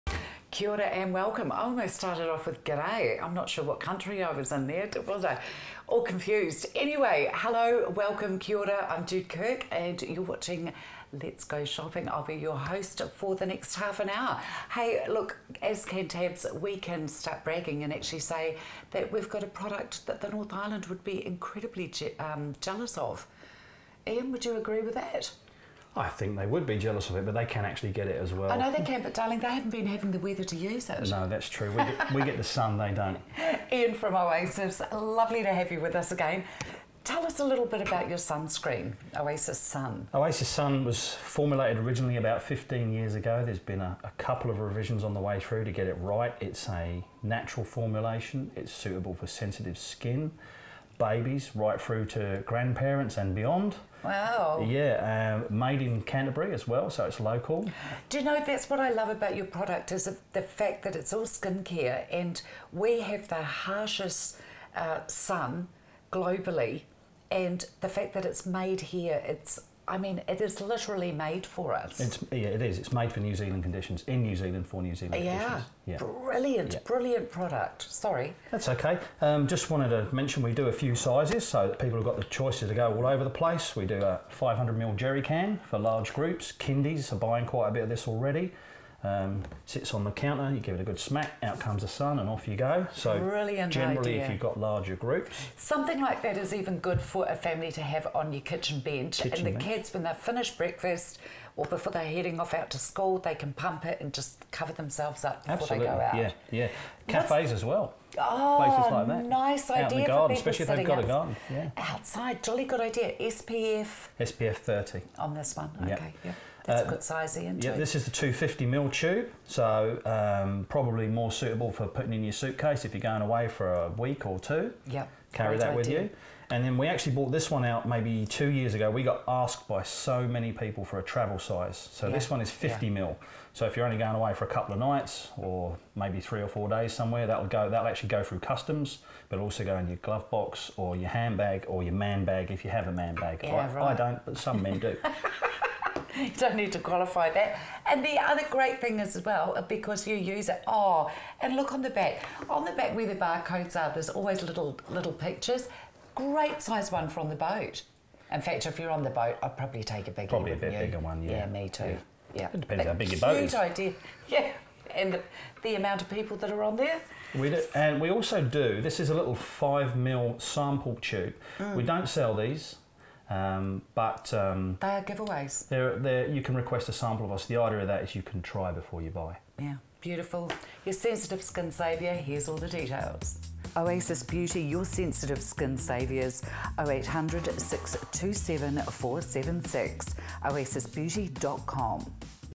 Natural New Zealand sunscreen for sensitive skin
Oasis Sun SPF 30 is New Zealand’s favourite healthy family sunscreen for sensitive skin.  Suitable for everybody, you can use it on your face and your body with confidence.

The UV conditions in New Zealand are proven to be the worst in the world.  Oasis Sun SPF 30 has been protecting New Zealanders from the harsh sunlight conditions for over 10 years - so it's a sunscreen you can trust.